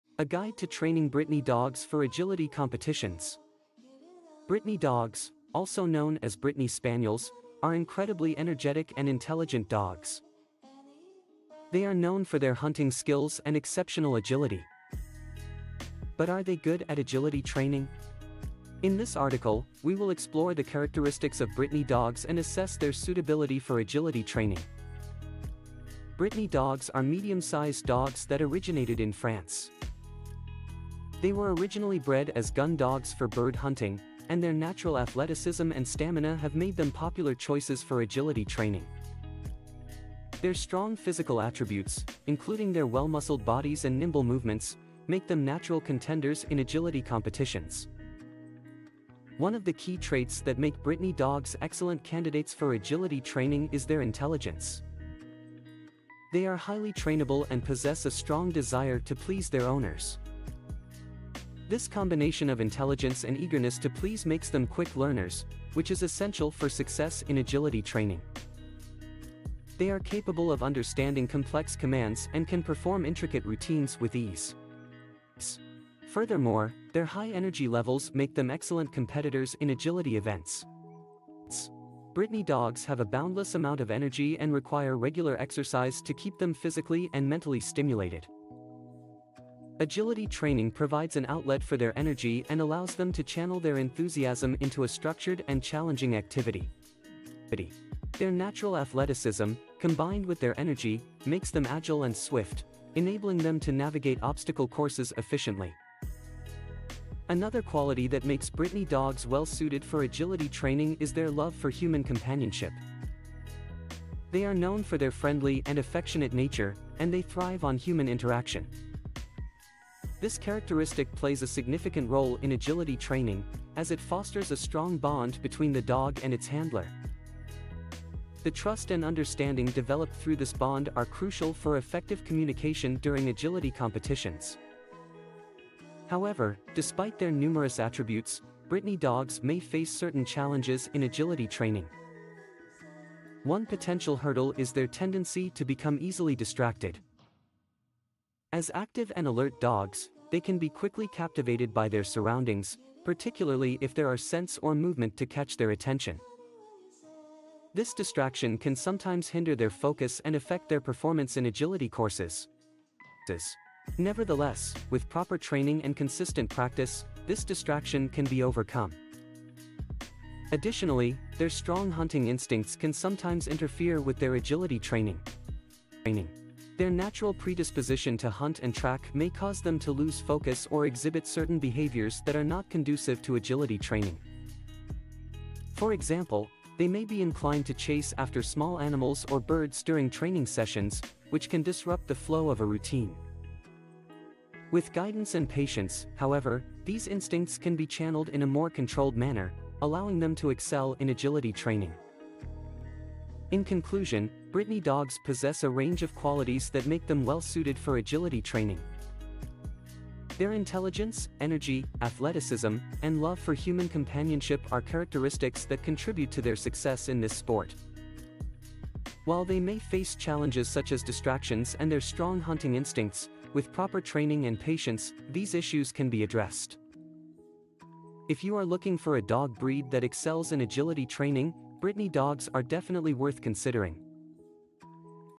A Guide to Training Brittany Dogs for Agility Competitions
Brittany dogs, also known as Brittany Spaniels, are incredibly energetic and intelligent dogs  They are known for their hunting skills and exceptional agility  But are they good at agility training? In this article, we will explore the characteristics of Brittany dogs and assess their suitability for agility training

Brittany dogs are medium-sized dogs that originated in France  They were originally bred as gun dogs for bird hunting, and their natural athleticism and stamina have made them popular choices for agility training
00:00 A Guide to Training Brittany Dogs for Agility Competitions